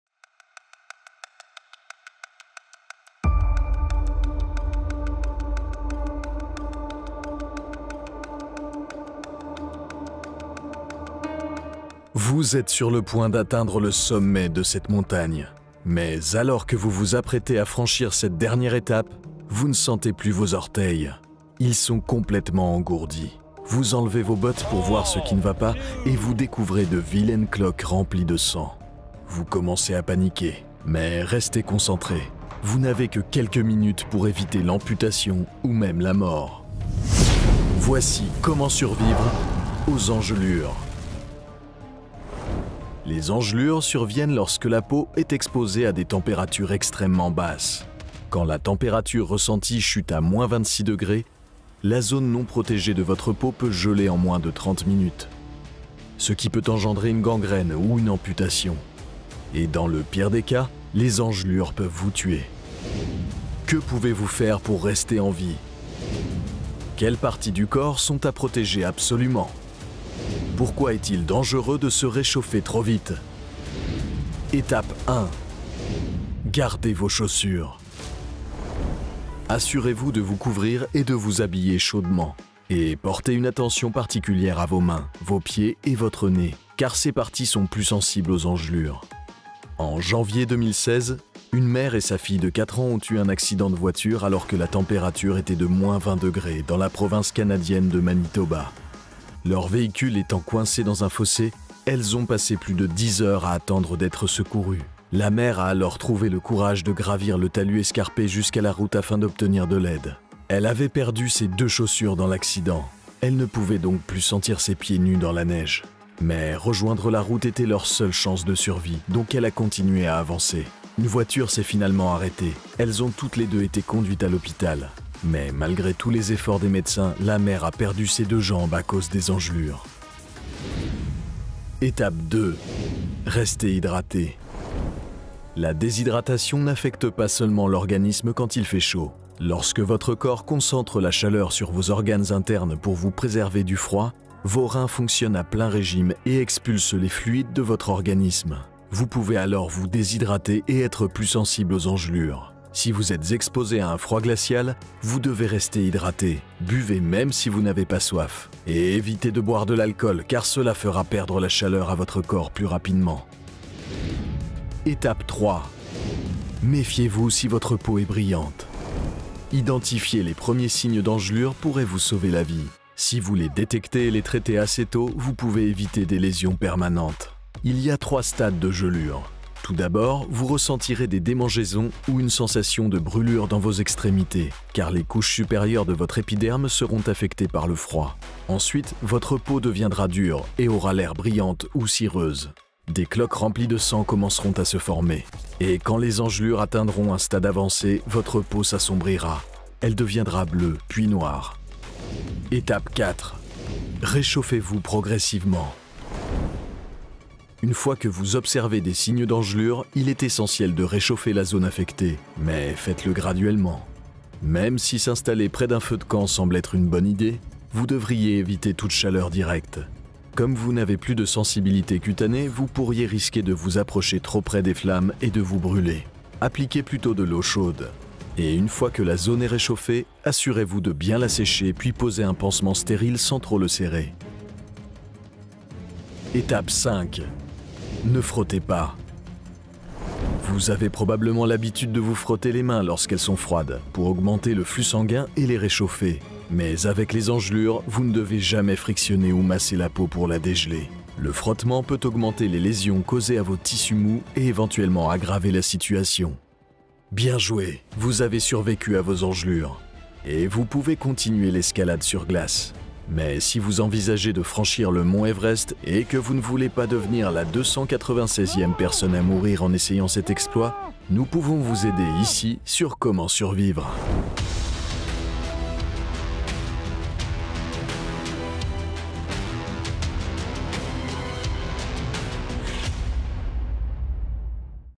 Comment survivre aux engelures
Les engelures surviennent lorsque la peau est exposée à des températures extrêmement basses. Quand la température ressentie chute à –26 °C, la zone non protégée de votre peau peut geler en moins de 30 minutes, ce qui peut engendrer une gangrène ou une amputation. Et dans le pire des cas, les engelures peuvent vous tuer. Quelles parties du corps sont à protéger absolument ? Pourquoi est-il dangereux de se réchauffer trop vite ?